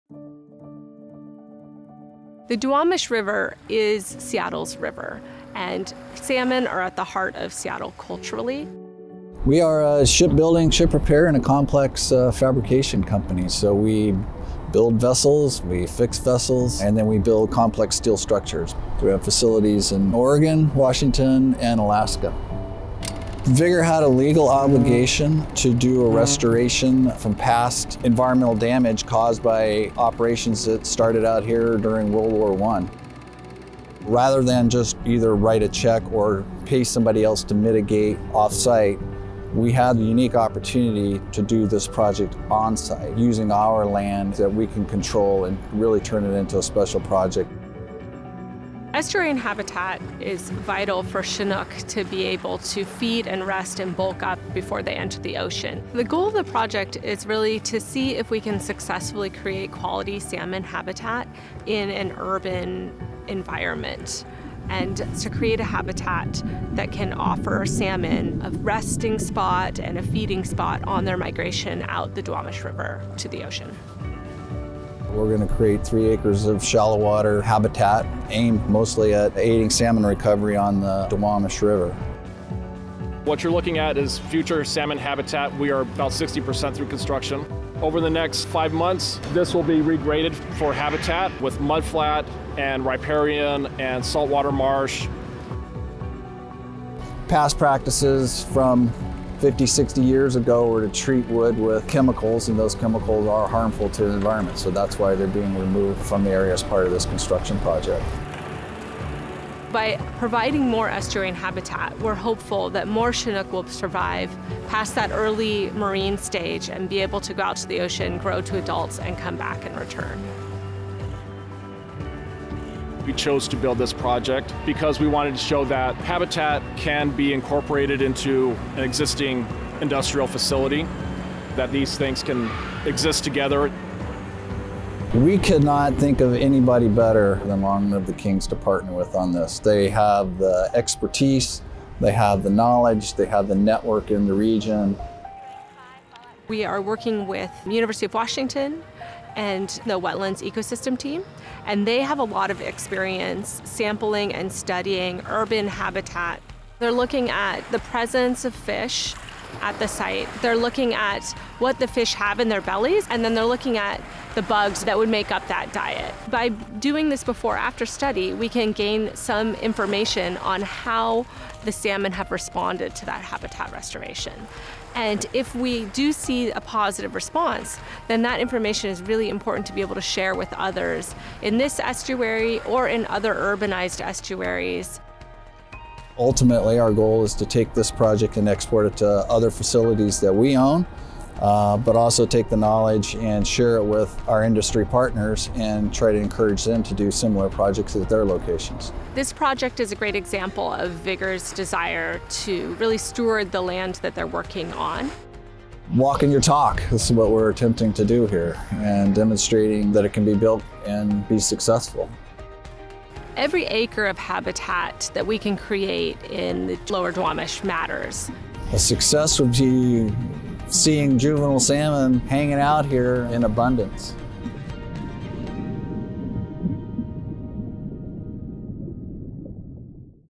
Urban Estuary Restoration at Vigor
Can a working shipyard in the heart of Seattle's industrial waterfront offer healthy habitat for juvenile salmon? At their Harbor Island facility, Vigor is constructing 2.6 acres of estuary habitat to support Duwamish River salmon on their migration to the ocean. Long Live the Kings and the UW's Wetland Ecosystem Team are partnering to monitor the site before and after restoration, gathering data for what we hope will be a model for the region.

See a day in the life of an aquatic scientist with the UW Wetland Ecosystem Team

Lower Duwamish aerial footage by Lighthawk.